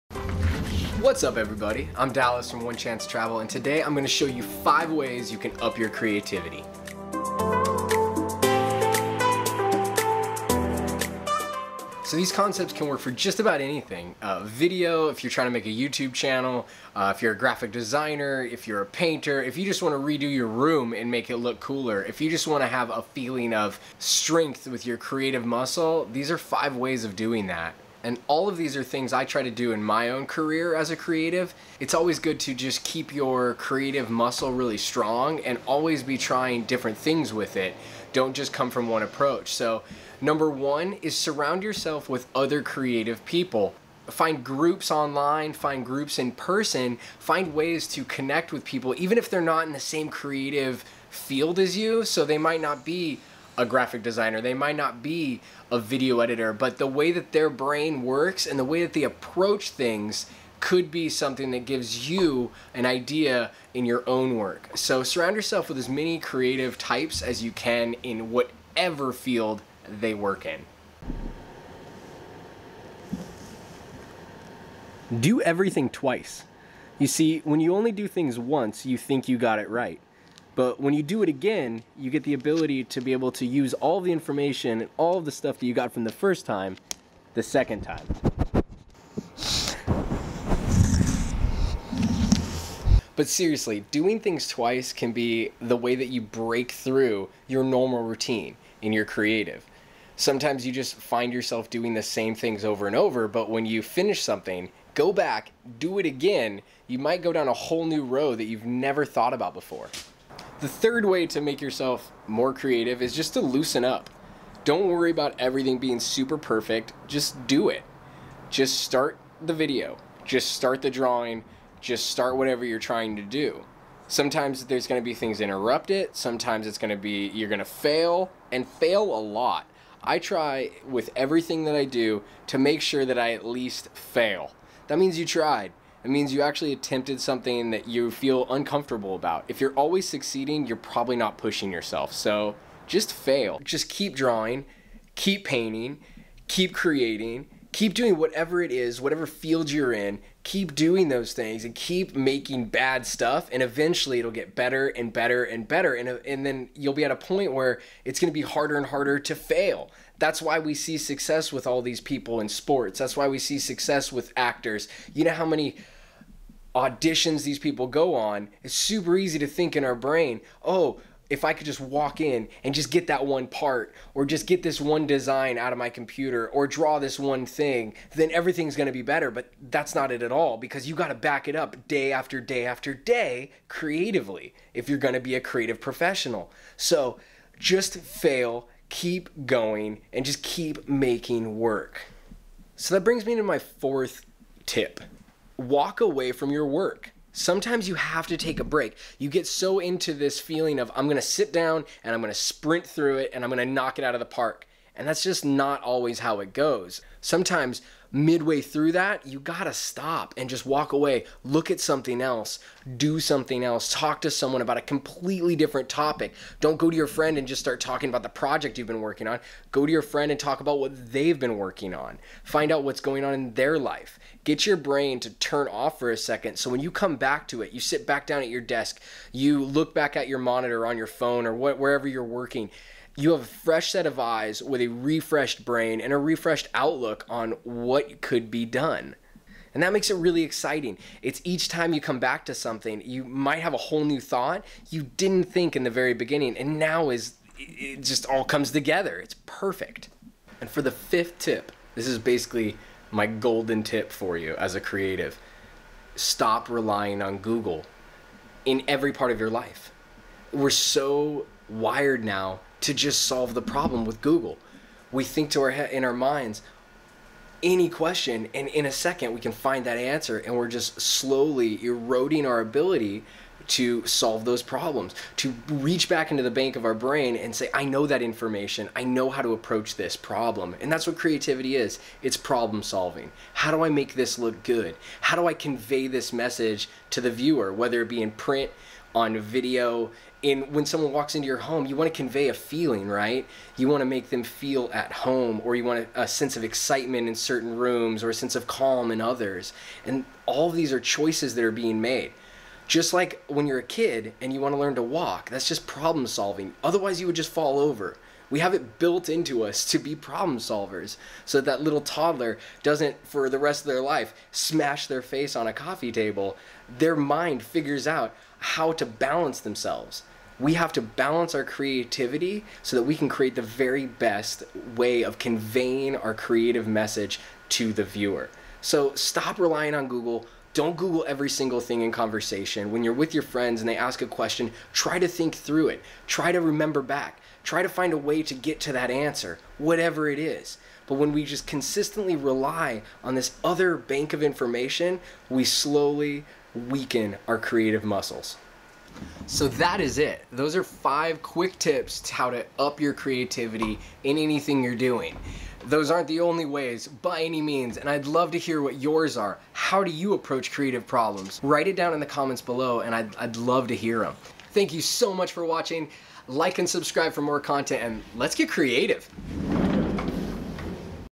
5 FAST ways to INCREASE your CREATIVITY
Here are five ways I've found to strengthen your creative muscles.

No matter if you're a graphic designer, video editor, painter, home designer, cake decorator or any other type of creative professional or amateur there are always ways to improve!

How do you become more creative? What approaches to do you take when making your work?

Our Kit
The Main Camera
Our Waterproof Camera
Custom Camera Bag
Our Gimbal